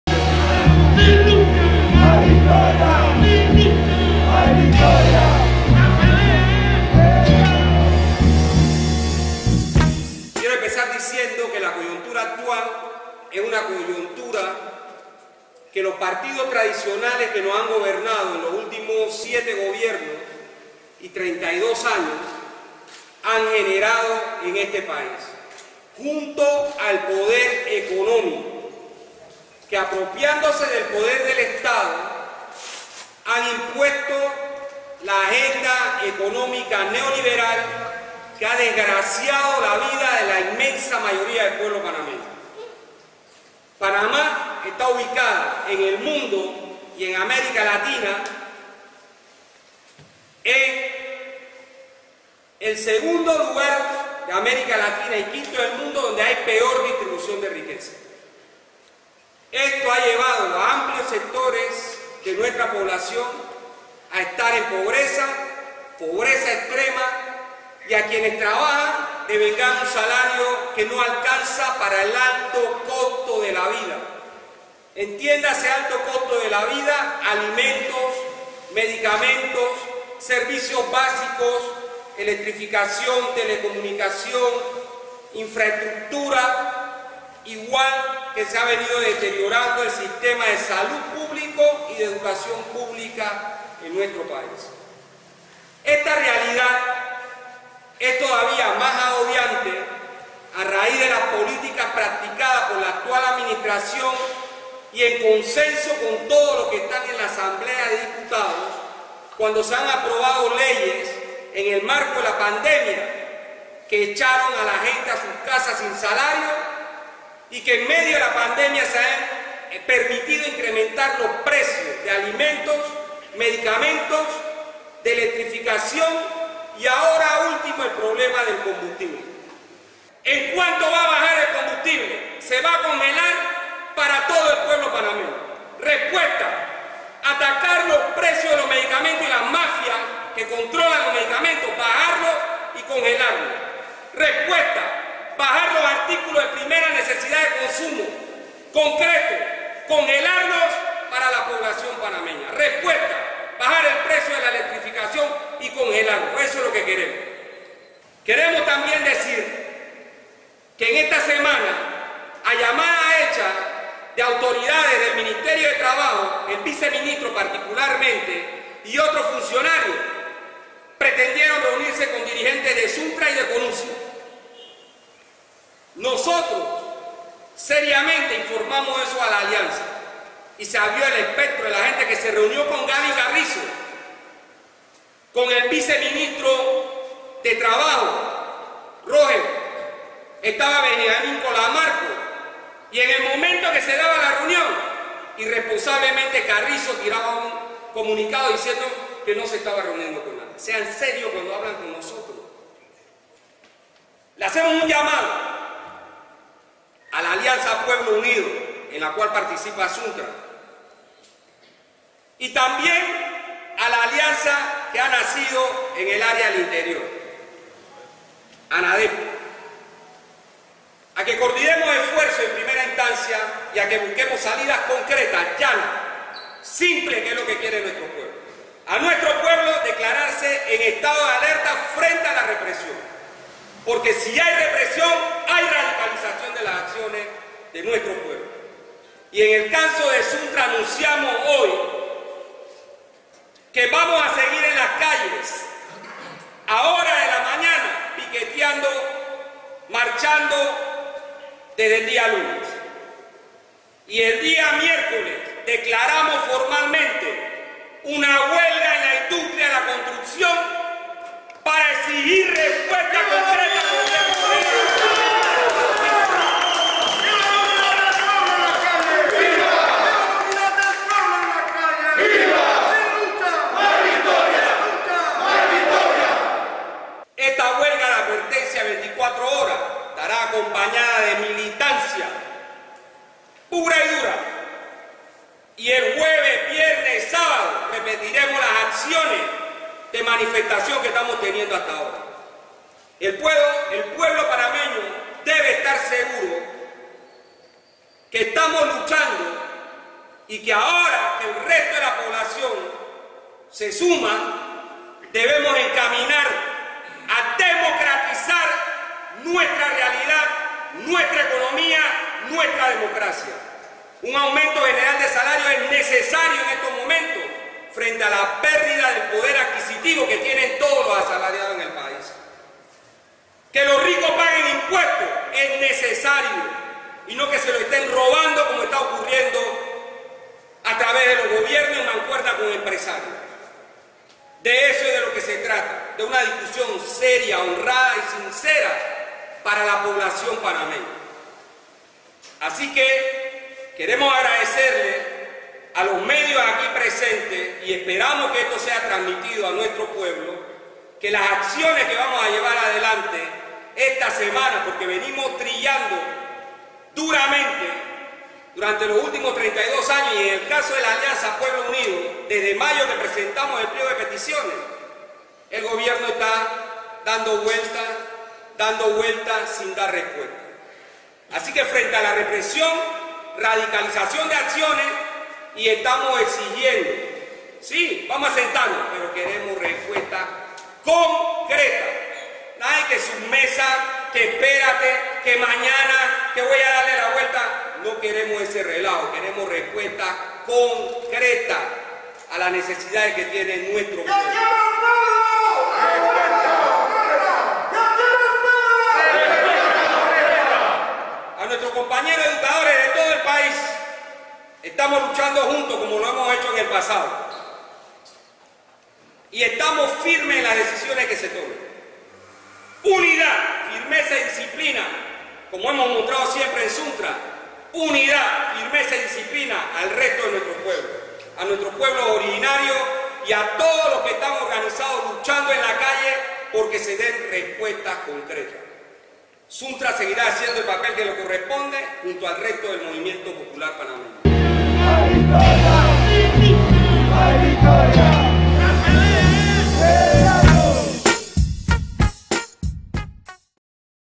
SUNTRACS anuncia huelga nacional de advertencia de 24 horas 1072022
Huelga nacional de advertencia de 24 horas a partir de este miércoles 13 de julio.
Siguen protestas y piqueteos.
Marcha nacional martes 12 de julio 4:00 pm parque Porras.